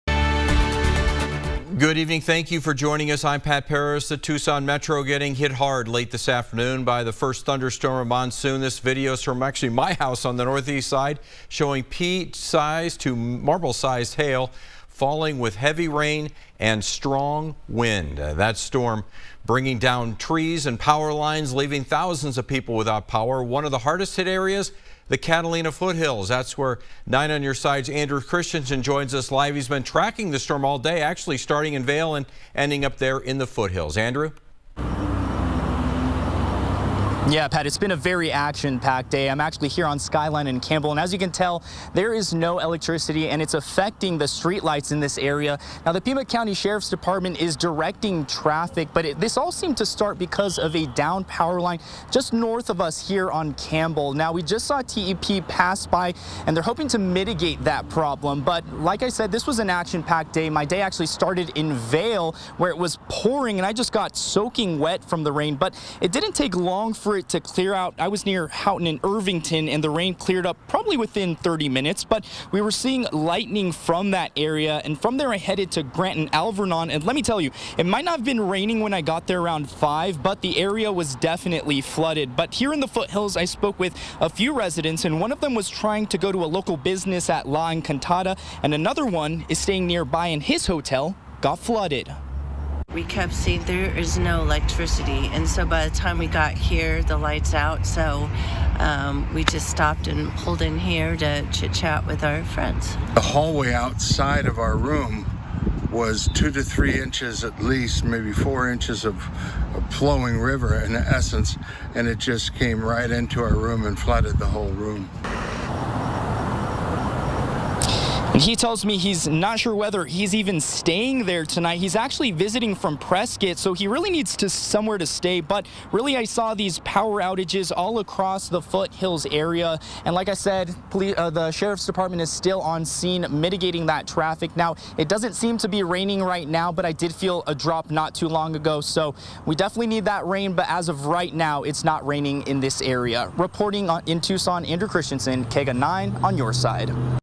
Tucson First Monsoon of 2023
The Tucson Electric Power is continuing to repair connections to homes as thousands of customers are without electricity.